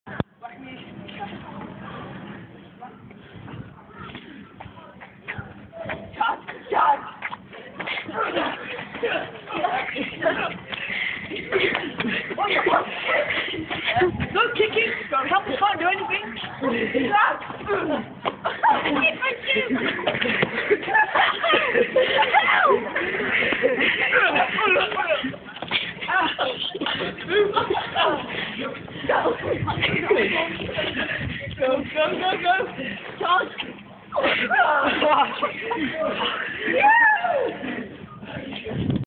piggy back fight
a mad fight. jus my m8s muckin around